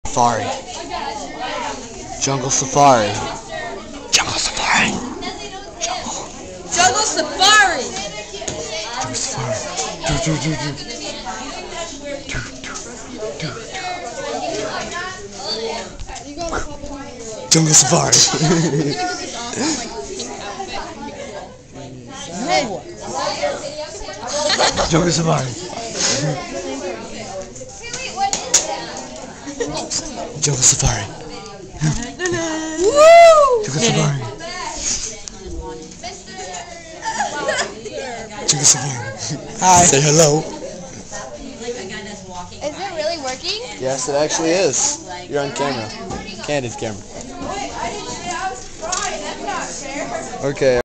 Classroom Safari
this is a video that a kid in my group for 4th hour named chanler filmed with the camera we needed for a project